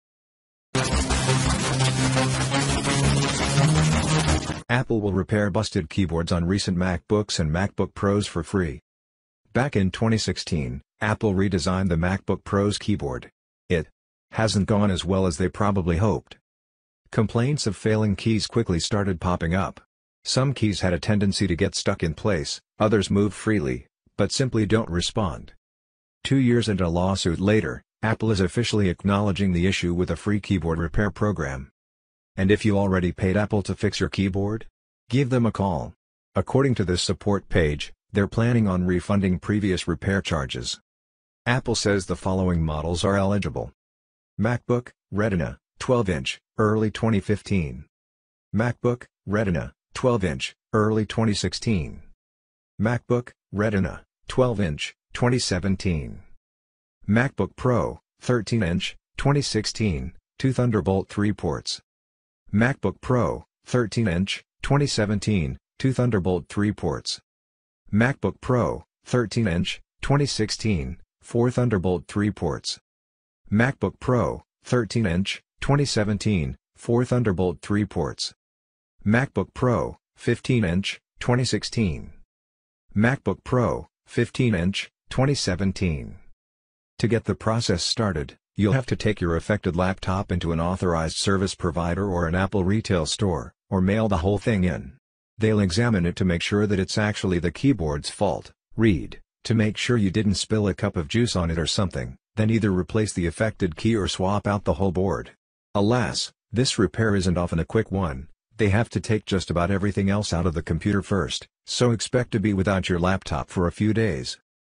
'Apple' THAT'S SOME NICE GESTURES Find out what new policy Apple is applying for its customers here
Apple will repair busted keyboards on recent MacBooks and MacBook Pros for free In 2016, Apple redesigned the keyboard of the MacBook Pro. It has not gone as well as they probably expected.

Complaints about key failures began to appear quickly. Some keys had a tendency to get stuck in their place; others move freely, but they simply do not respond.

Two years and a subsequent lawsuit, Apple officially recognizes the problem with a free keypad repair program.

What if you already paid Apple for fixing your keyboard? Call them. According to this support page, they plan to reimburse the previous repair charges.

Apple says that the following models are eligible:

MacBook (Retina, 12 inches, early 2015)
MacBook (Retina, 12 inches, early 2016)
MacBook (Retina, 12 inches, 2017)
MacBook Pro (13 inches, 2016, two Thunderbolt 3 ports)
MacBook Pro (13 inches, 2017, two Thunderbolt 3 ports)
MacBook Pro (13 inches, 2016, four Thunderbolt 3 ports)
MacBook Pro (13 inches, 2017, four Thunderbolt 3 ports)
MacBook Pro (15 inches, 2016)
MacBook Pro (15 inches, 2017)
To begin the process, you will need to take your affected laptop to an authorized service provider or an Apple retail store, or mail it. They will examine it to make sure it's really the keyboard's fault (read: to make sure you do not spill a cup of juice on it or something), then replace the affected key or change the entire table. Unfortunately, this repair is usually not fast (first you have to remove almost everything else from the computer), so expect to be without your laptop for a few days.